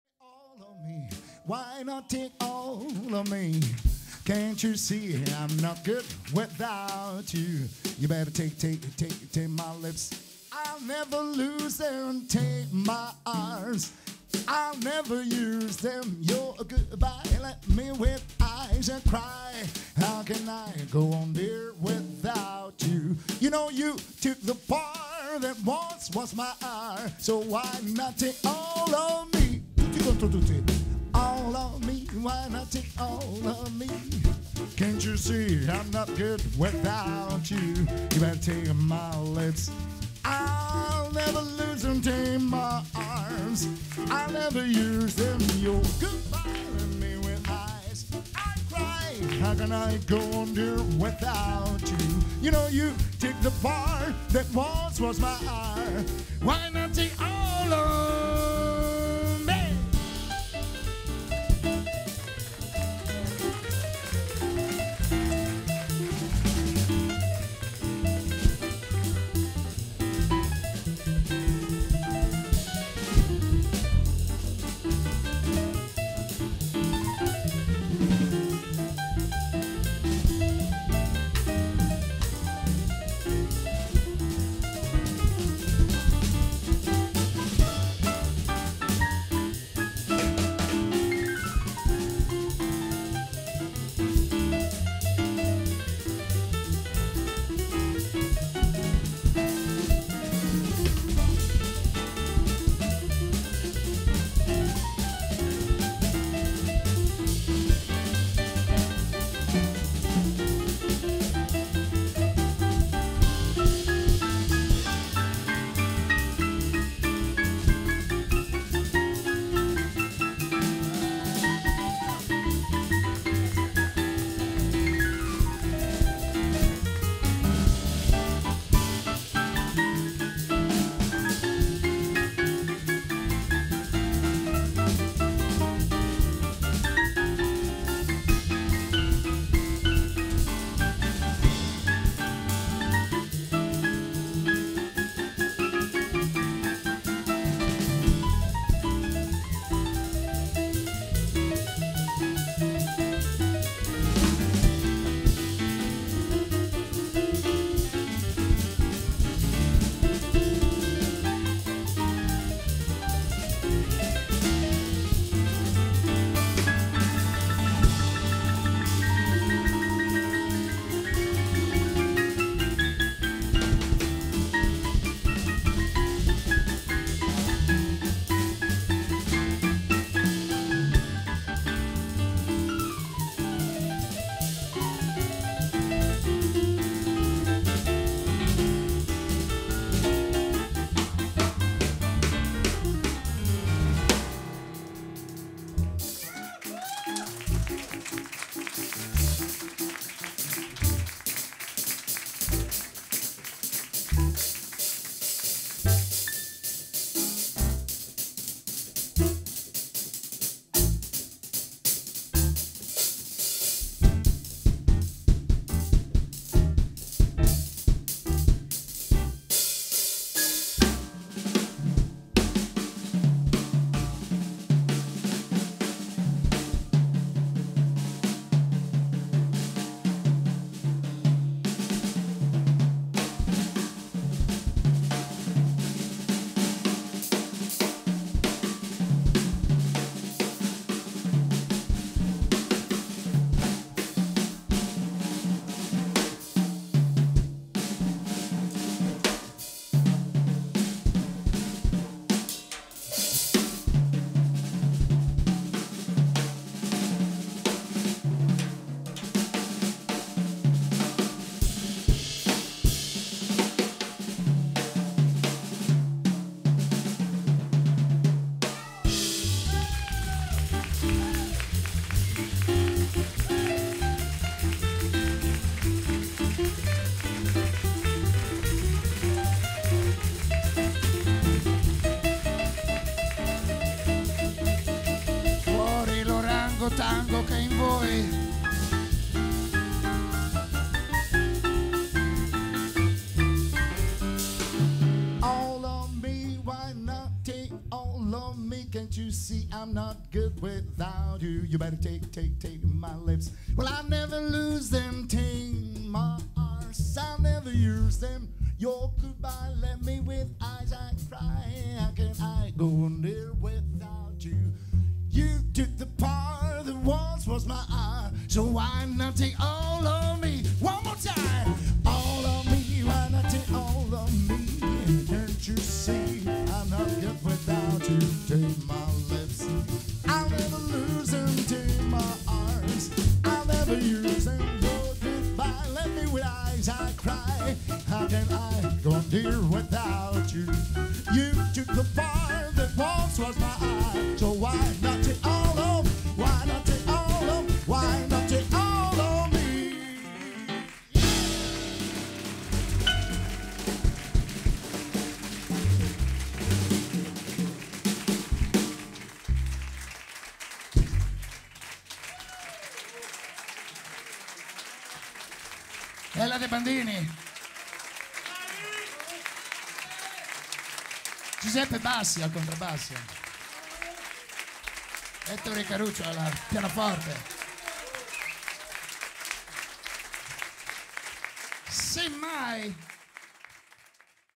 Alan Farrington_Ellade Bandini 4tet @ Moody Jazz Cafe - Foggia - 29/03/2010
Alan Farrington - vocal / Ettore Carucci - piano / Giuseppe Bassi - bass / Ellade Bandini - drums